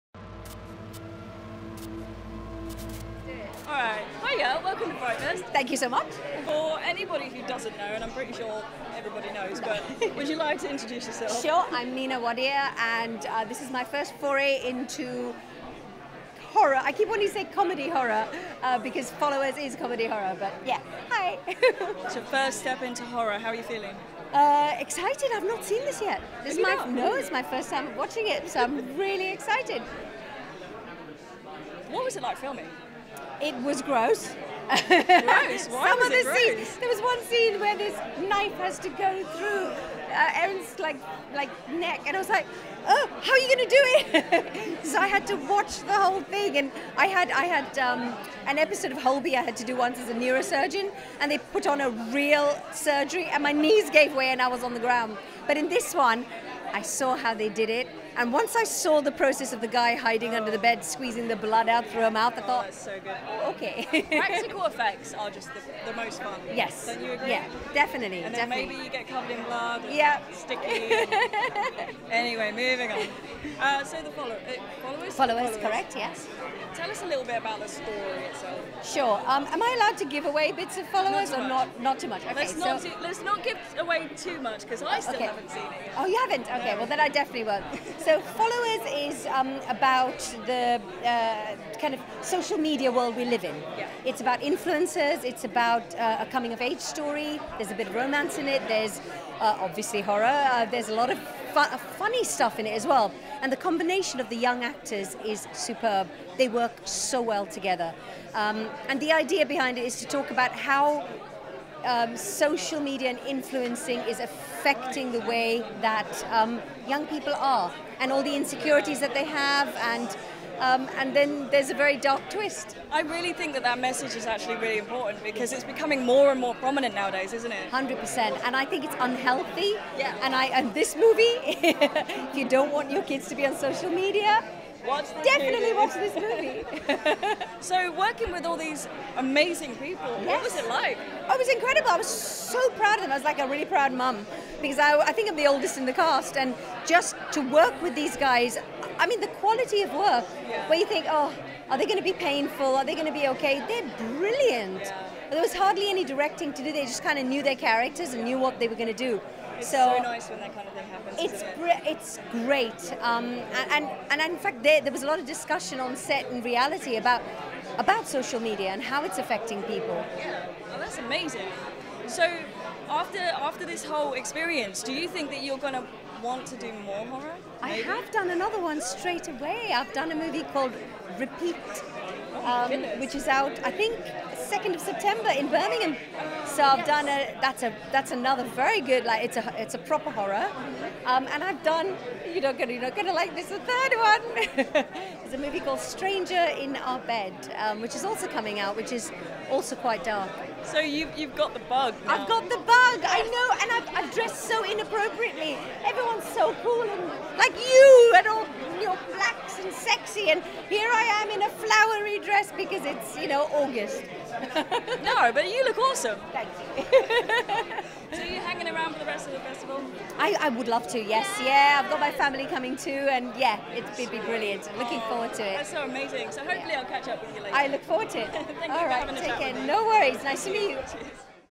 Red Carpet with Nina Wadia OBE, FrightFest 2021
Hosted by Luna Wolf

FrightFest TV crew:
Camera Op: Charlotte Noble
Editor: Matthew Crossley
Producer: Mark Scullion